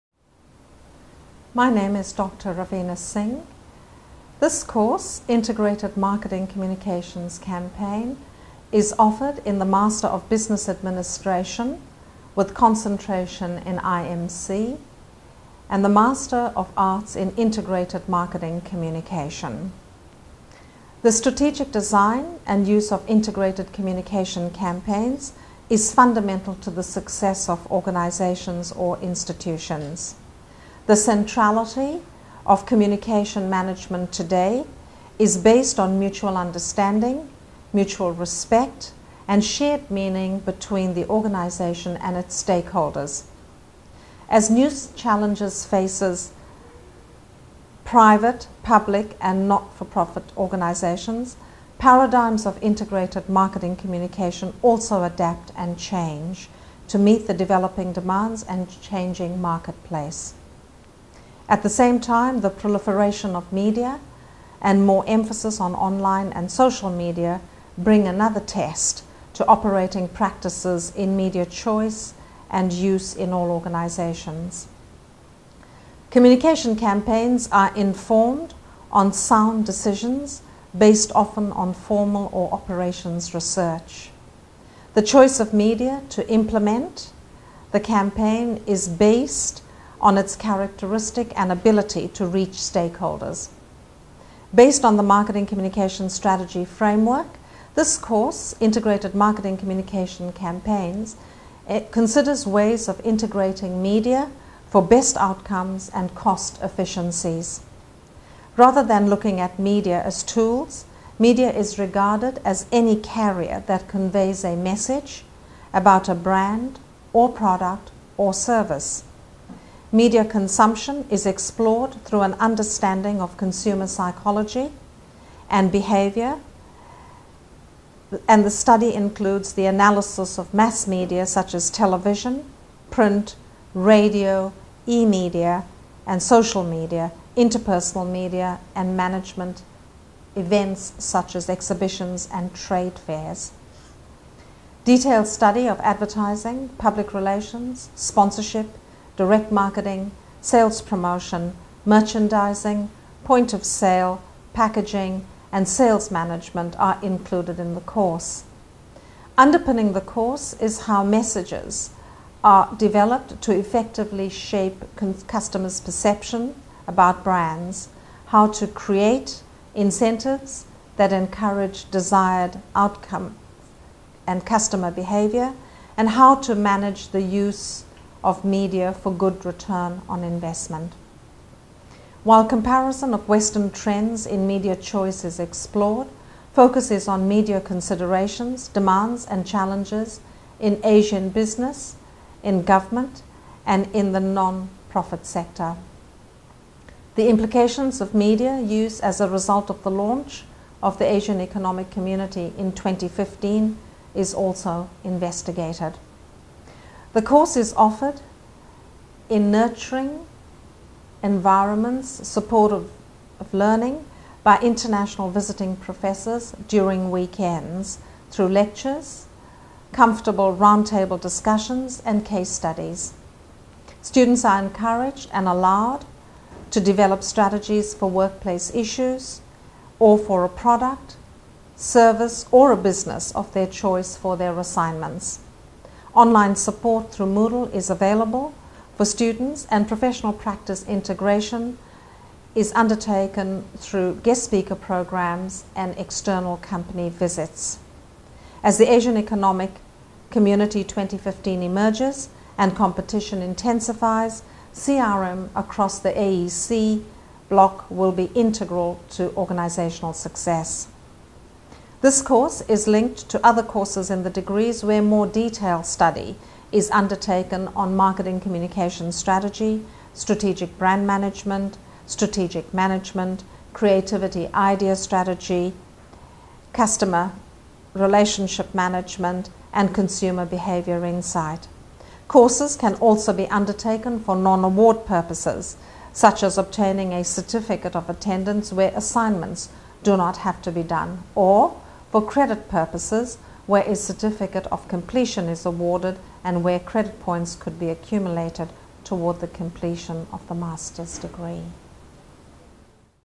Dr. Raveena Singh, Visiting Professor at DPUIC
Dr. Raveena Singh, Visiting Professor
Course Title: Integrated Marketing Communications Campaigns
Programs:
Master of Business Administration with Concentration in Integrated Marketing Communications (MBA in IMC)
Master of Arts in Integrated Marketing Communications (MA IMC)
University: Dhurakij Pundit University International College (DPUIC)